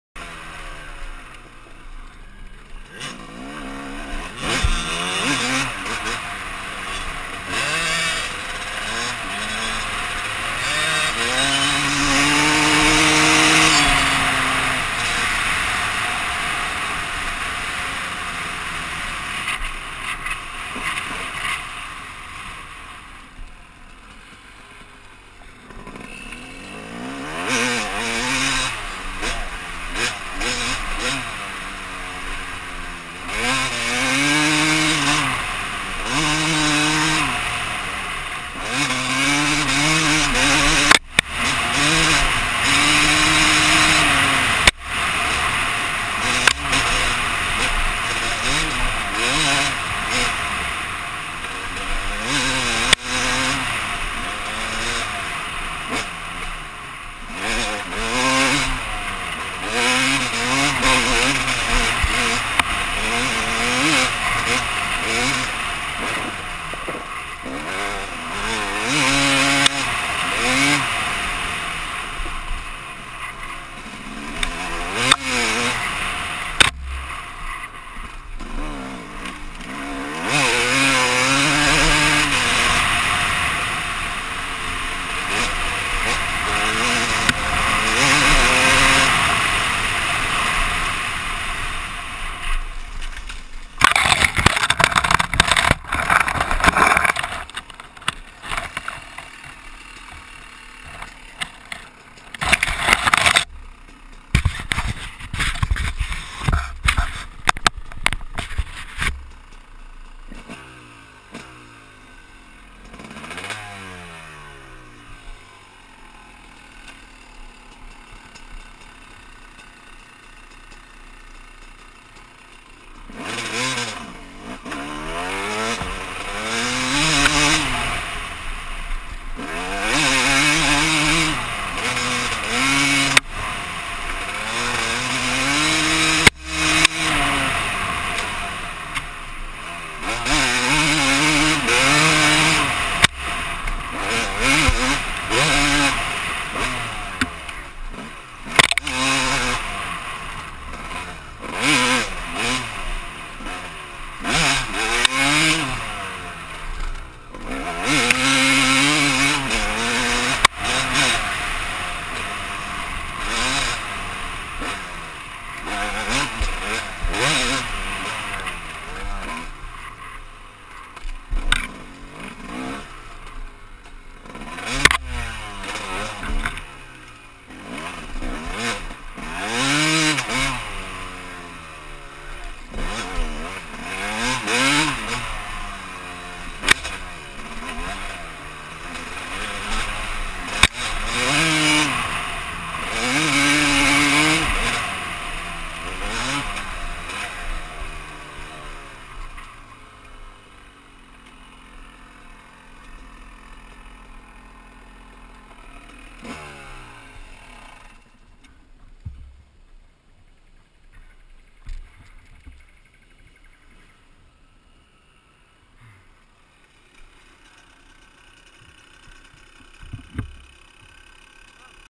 Quick Loop
Quick loop in the forestry, riding a yz250